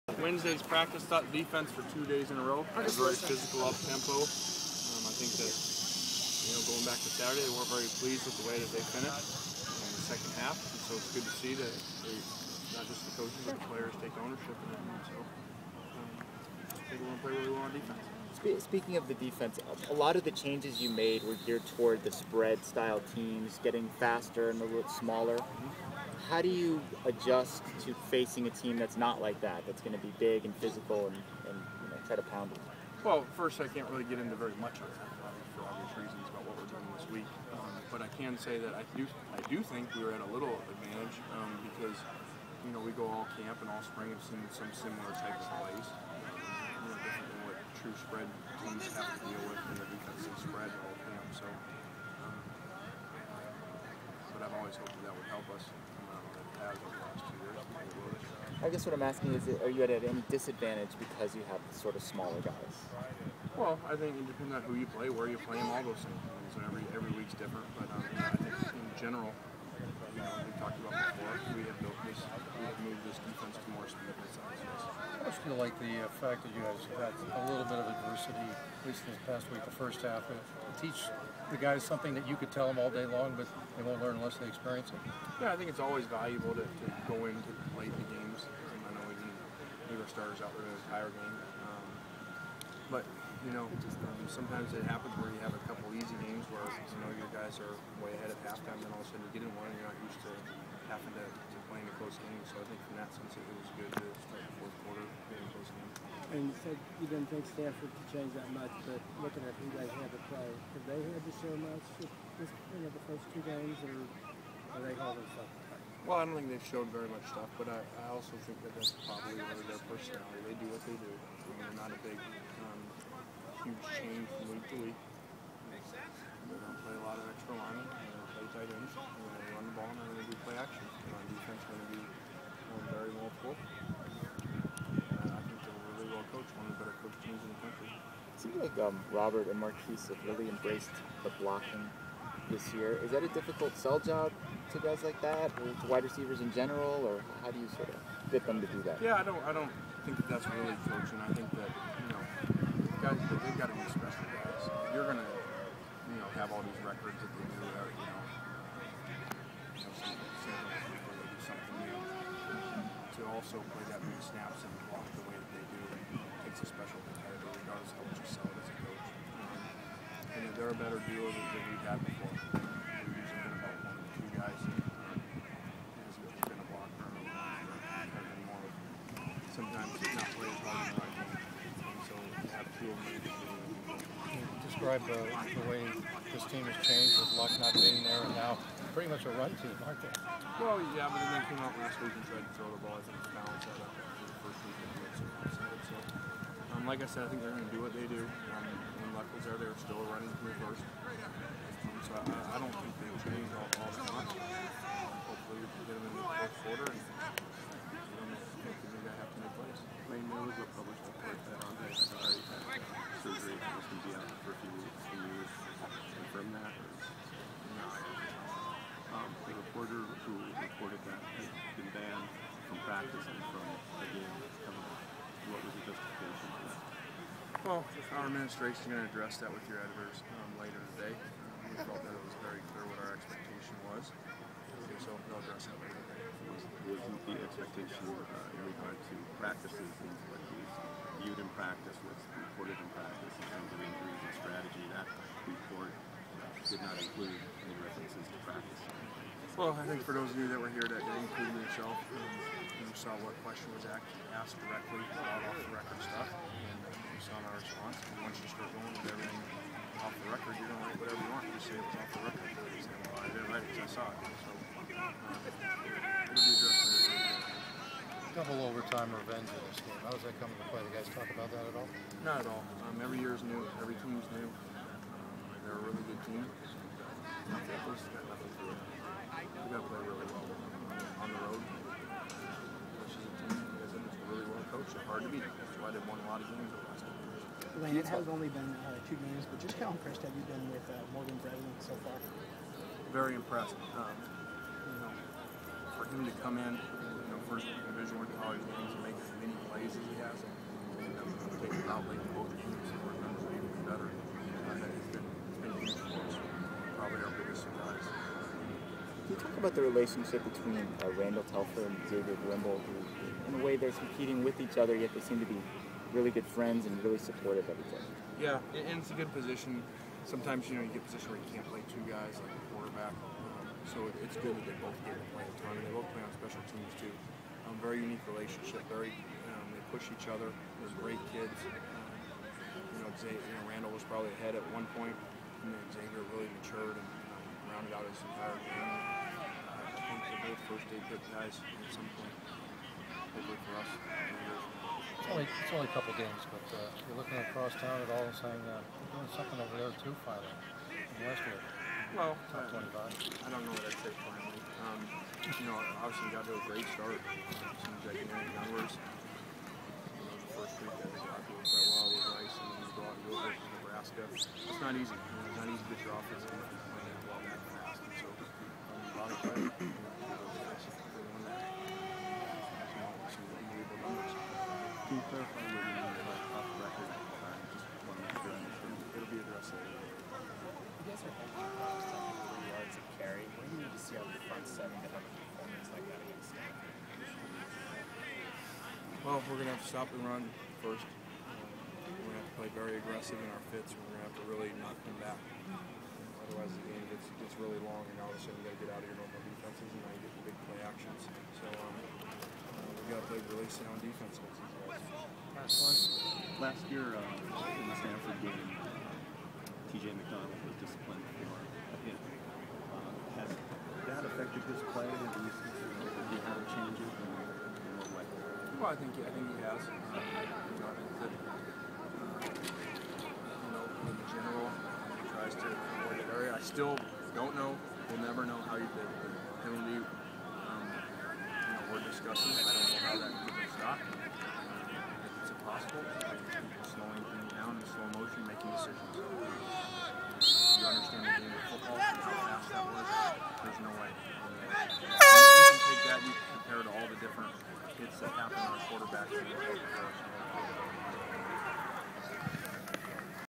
Lane Kiffin Press Scrum: Sept. 12, 2012
USC head coach Lane Kiffin talks to the media prior to facing Stanford